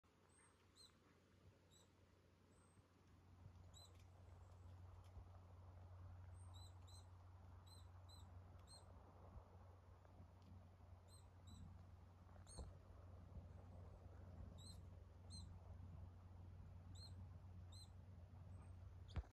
Gransångare, ssp tristis
Skumt lock, någon hade fotat en tristis där tidigare och den lät precis så här